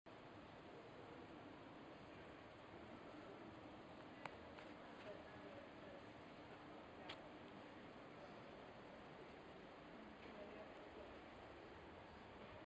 Paradoxymoron
3D Artwork at the British Library in London called Paradoxymoron by Patrick Hughes